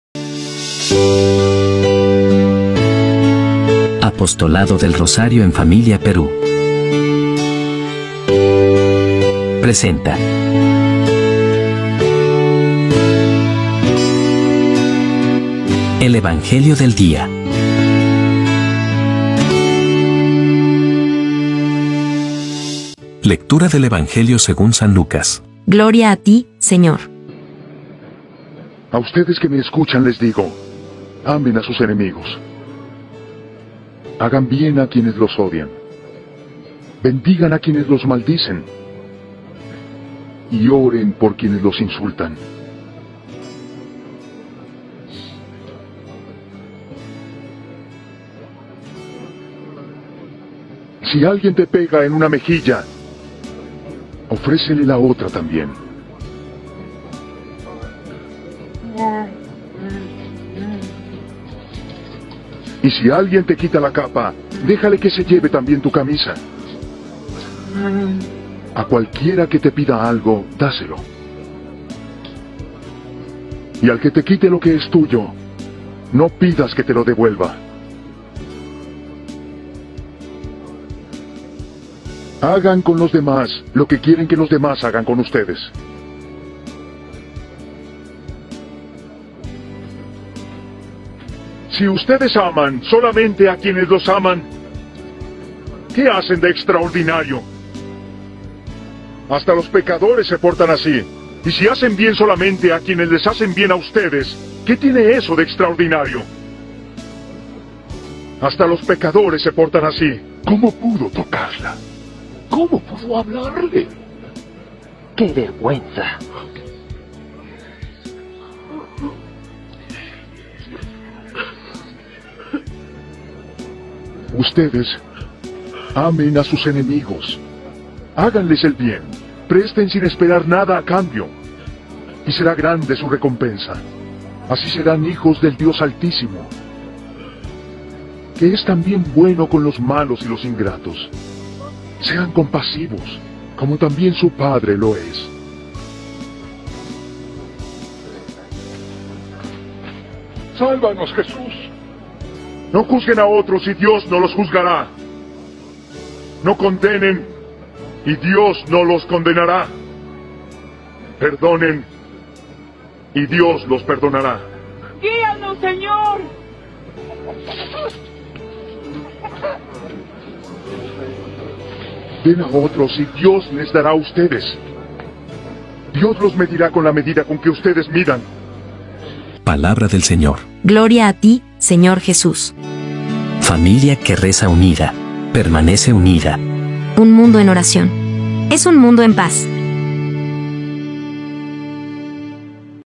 Evangelio de hoy - Jueves 14 de setiembre de 2023
Jueves 14 de setiembre de 2023
Lectura del Santo Evangelio según Lucas 6, 27-38
FAMILIA QUE REZA UNIDA PERMANECE UNIDA
APOSTOLADO DEL ROSARIO EN FAMILIA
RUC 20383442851